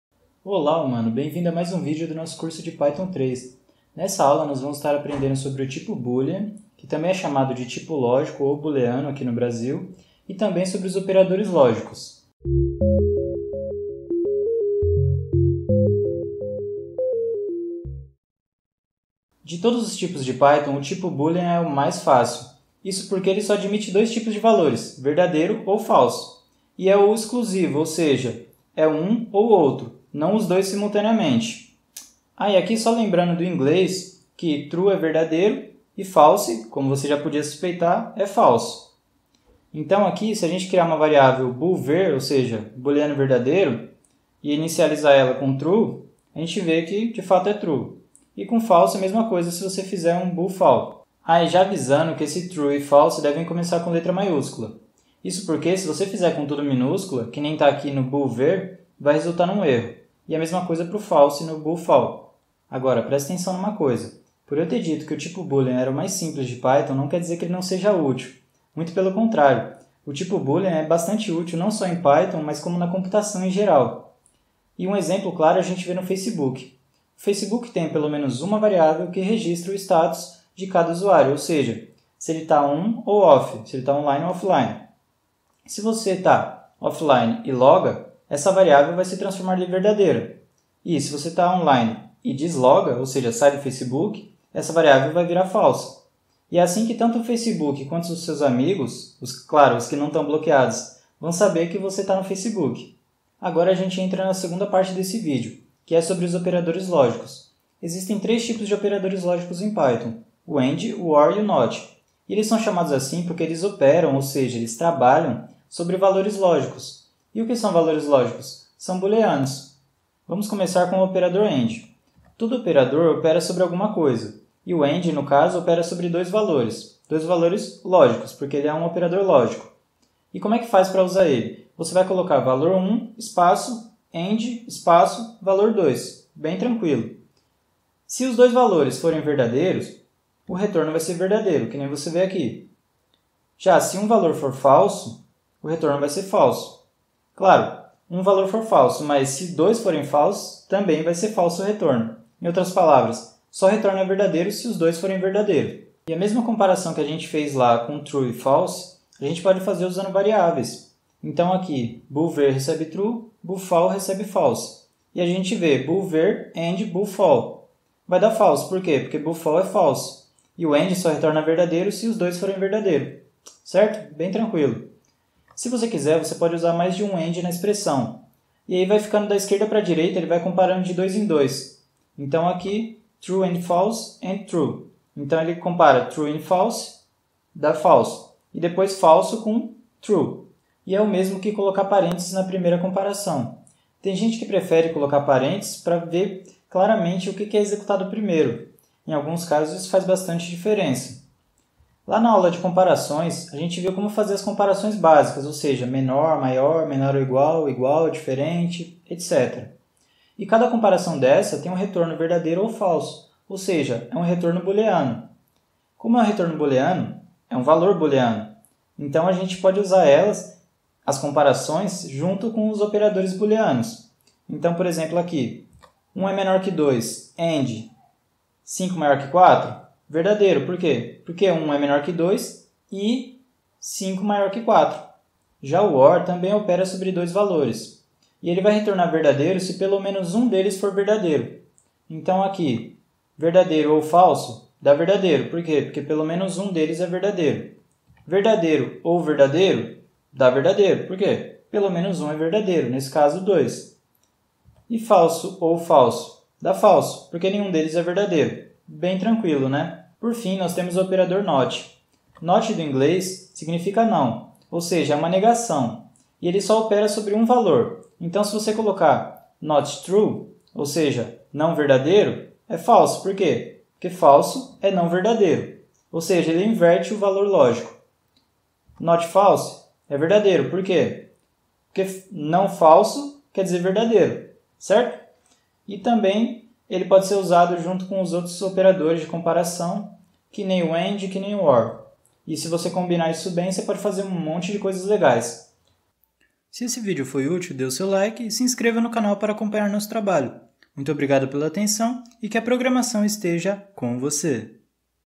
Curso Python 3 | Aula 11 - Tipo boolean e Operadores Lógicos
✅ APRENDA INGLÊS em uma das maiores escolas de idiomas do Brasil ➜ Olá, humano! Quer aprender Python?
Aqui apresentamos o tipo boolean e os operadores lógicos and, or e not.

Caio na Aula, criado por Caio Dallaqua, aluno de graduação em Física Computacional na USP (IFSC).